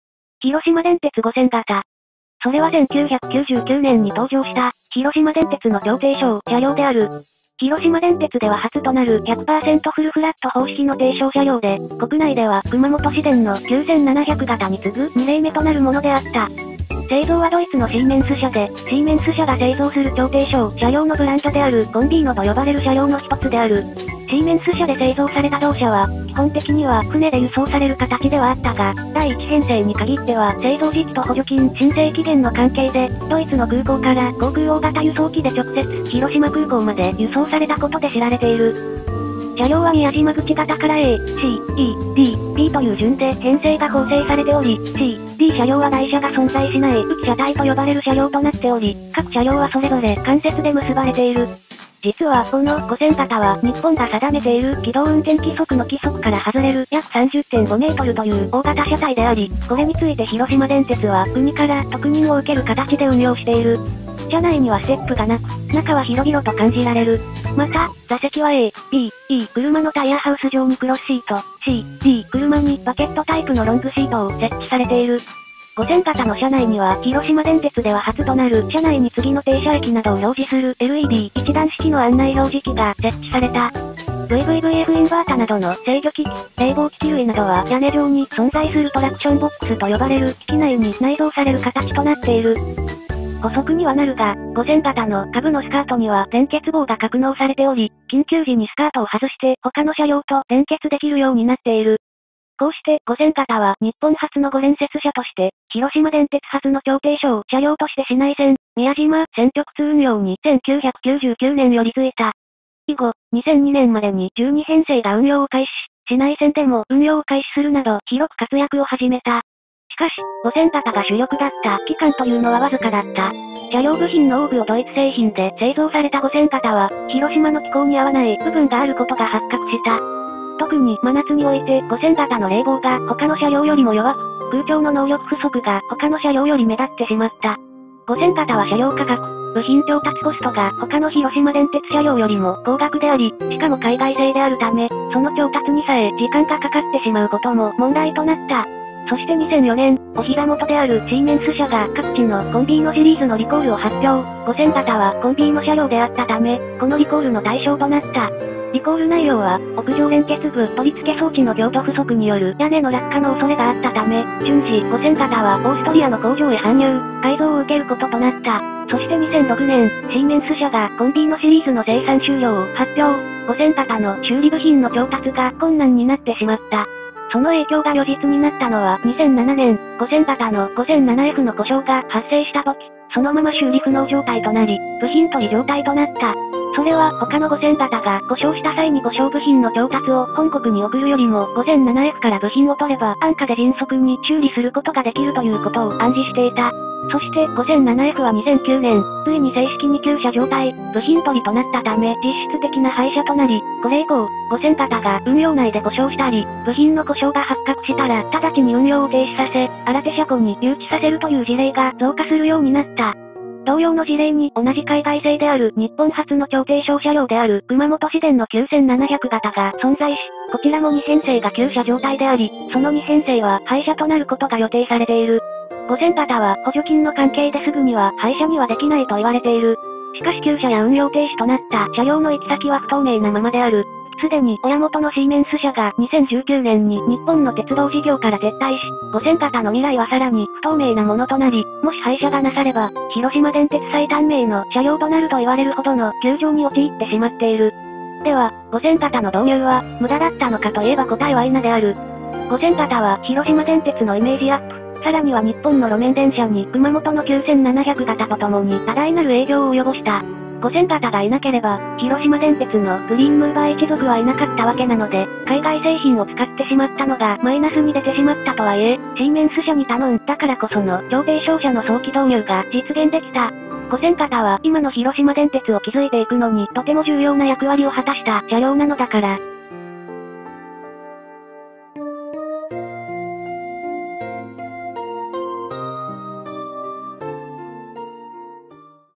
[鉄道車両解説] 日本に馴染めなかったドイツ生まれ 広島電鉄5000形とは？
実は10ヶ月ぶりの鉄道車両解説。
今回は広島電鉄初の超低床車両である，5000形のお話です。